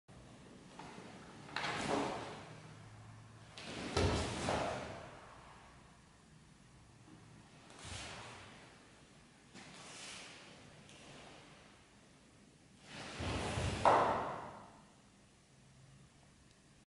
Iaido Art Ronin Art
mal eine gefilterte Aufnahme einer Ronin Kata am zweiten Tag des Tameshigiri - Fortgeschrittene